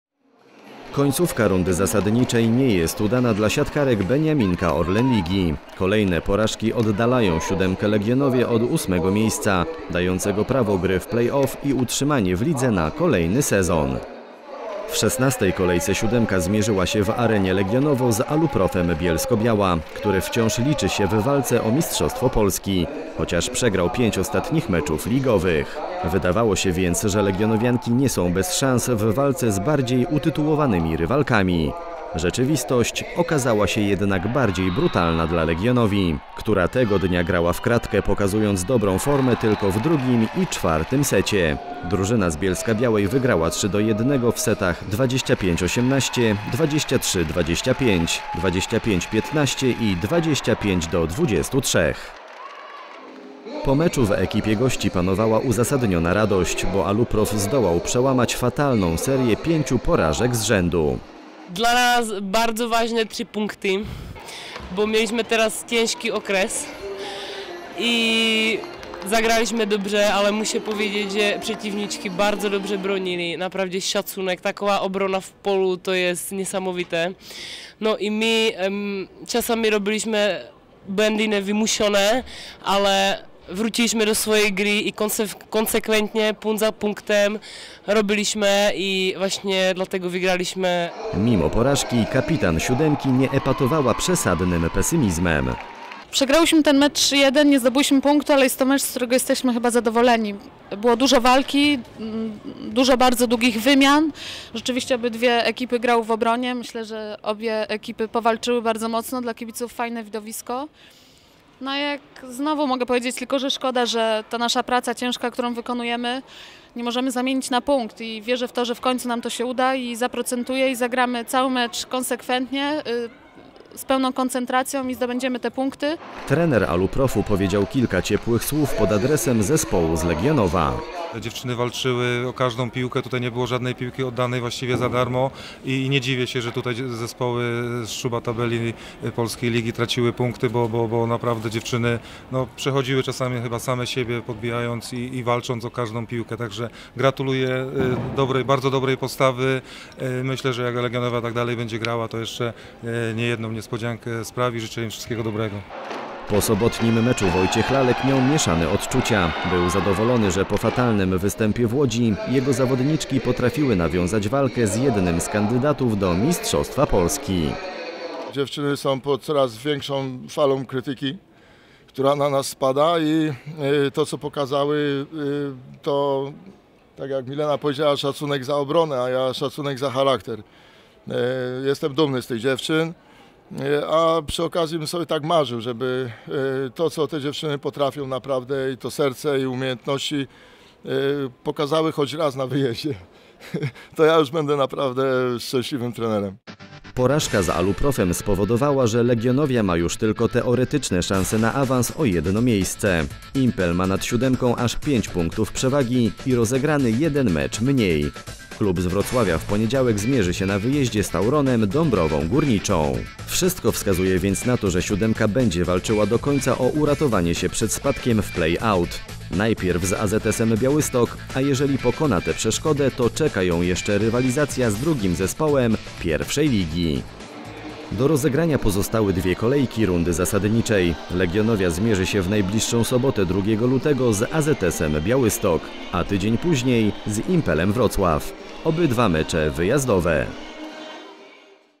Legionovia blisko play-out
Końcówka rundy zasadniczej nie jest udana dla siatkarek beniaminka Orlen Ligi. Kolejne porażki oddalają Siódemkę Legionovię od ósmego miejsca, dającego prawo gry w play-off i utrzymanie w lidze na kolejny sezon w extraklasie.
W szesnastej kolejce Siódemka zmierzyła się w Arenie Legionowo z Aluprofem Bielsko-Biala, który wciąż liczy się w walce o mistrzostwo Polski, chociaż przegrał pięć ostatnich meczów ligowych. Wydawało się więc, że legionowianki nie są bez szans, w walce z bardziej utytułowanymi rywalkami. Rzeczywistość okazała się jednak bardziej brutalna dla Legionovii, która tego dnia grała w kratkę, pokazując dobrą formę tylko w drugim i czwartym secie. Drużyna z Bielska-Białej wygrała 3:1 w setach 25:18, 23:25, 25:15 i 25:23.